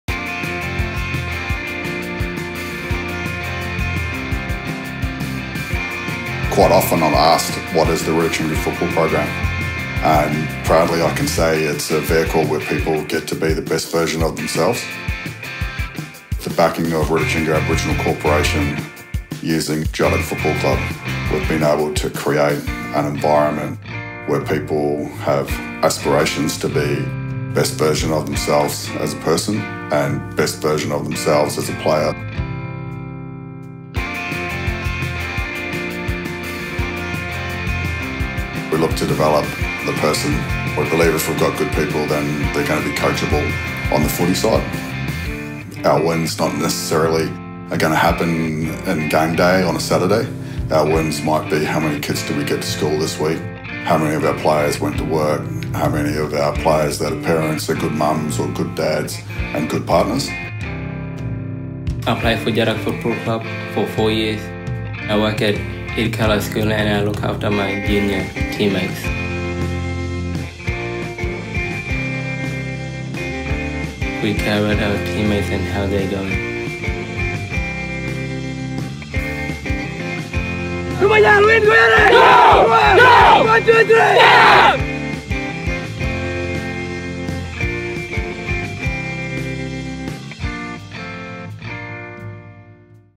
Djarrak Football Club [Official Video]
Rirratjingu Aboriginal Corporation use the Djarrak Football Club as a vehicle for positive social impacts across Yirrkala Community and the wider Gove Peninsula. Participants that adhere to strict behaviours on and off the field are exposed to opportunities including year round football in the Gove AFL and also the NTFL in Darwin.

Produced by Aris.